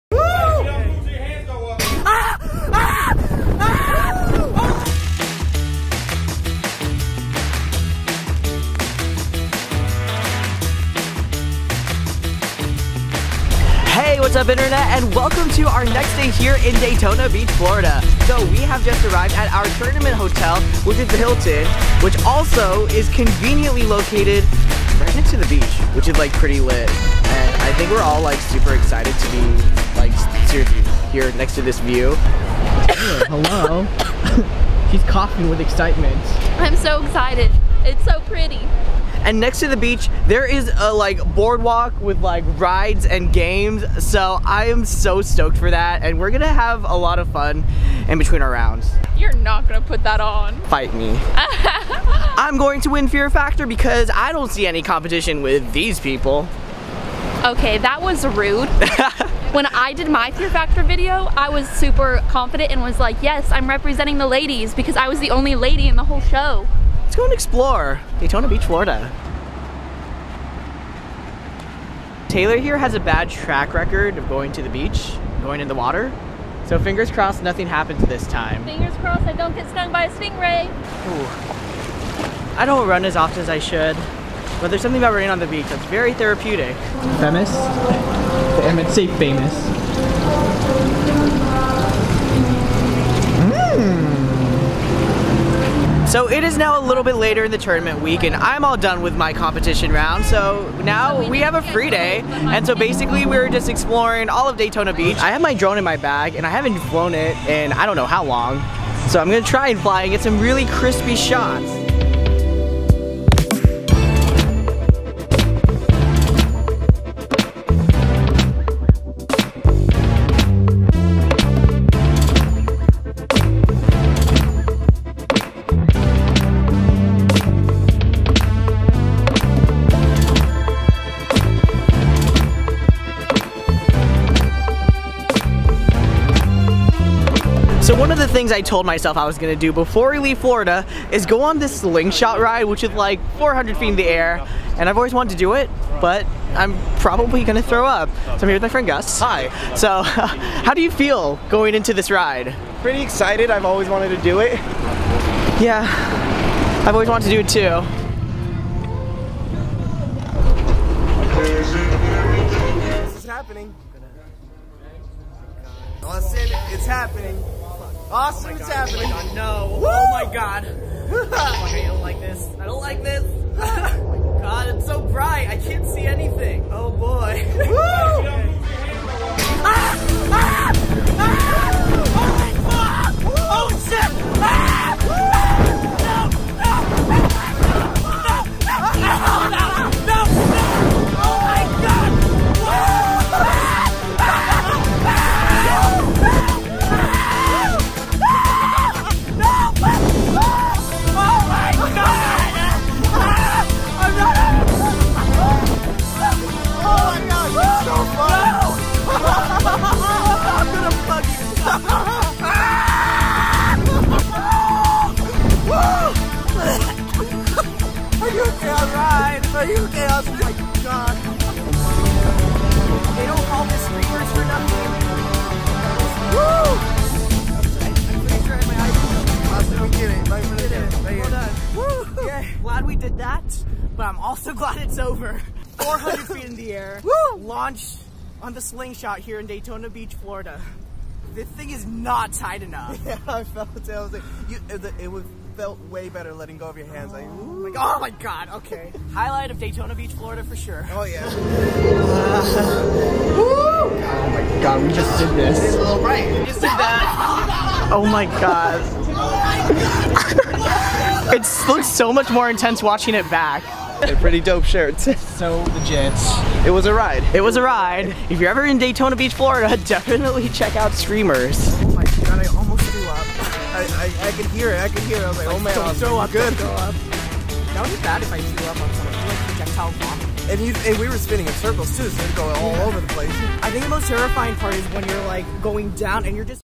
WORST FREAKOUT ON THE SLINGSHOT RIDE
While on our trip to Daytona Beach, Florida for Phi Rho Pi 2018, Gus and I decided to take a visit to Screamers for a shot at the Slingshot Ride!